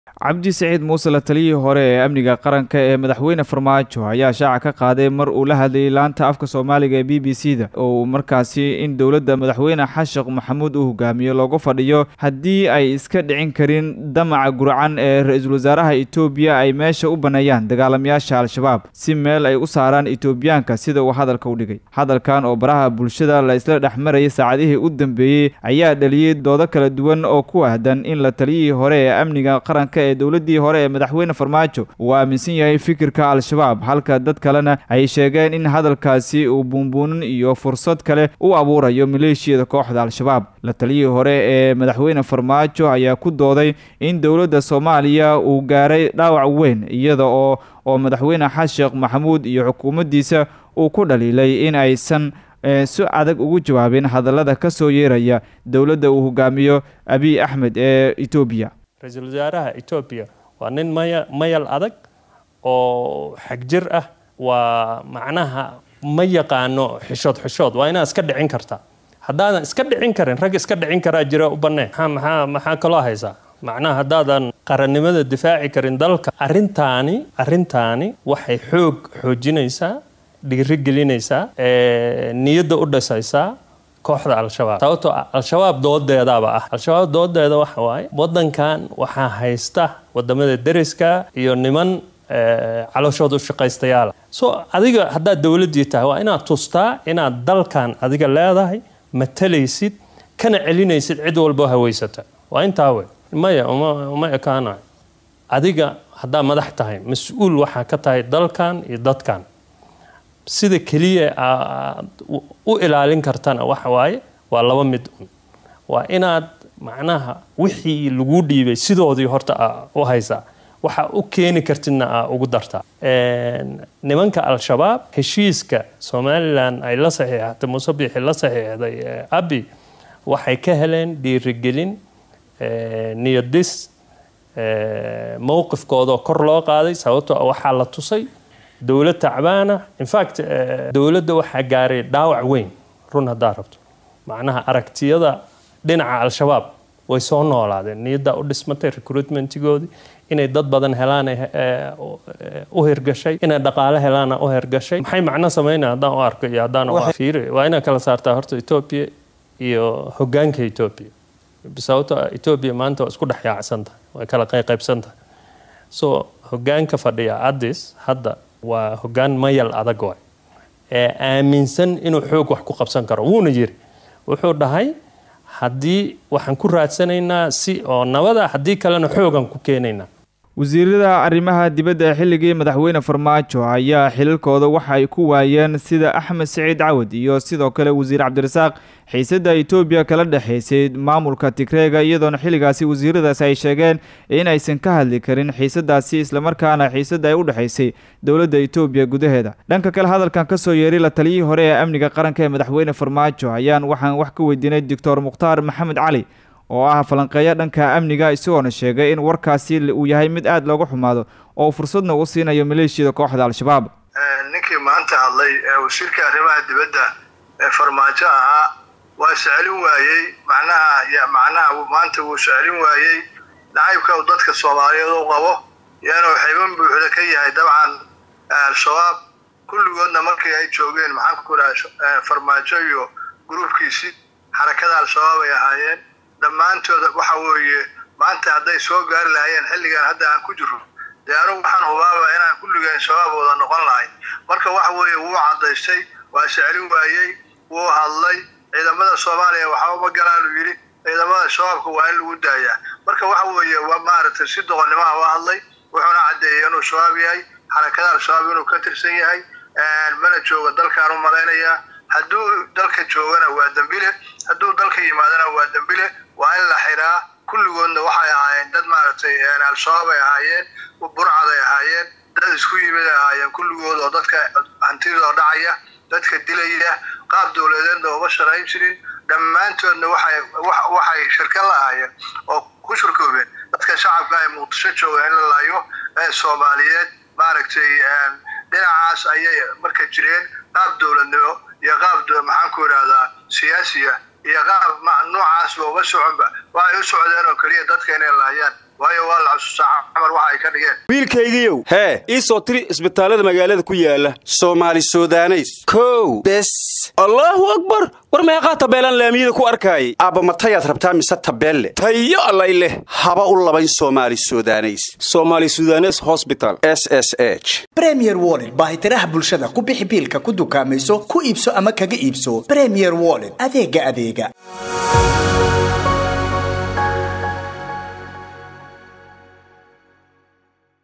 LA TALIYIHII HORE EE AMNIGA QARANKA MW FARMAAJO OO SHEEGAY IN DALKA LAGU WAREEJIYO AS SI LO.........
LA TALIYIHII HORE EE AMNIGA QARANKA MW FARMAAJO OO SHEEGAY IN DALKA LAGU WAREEJIYO AS SI LOOGA ADKAADO DAMACA ITOOBIYA.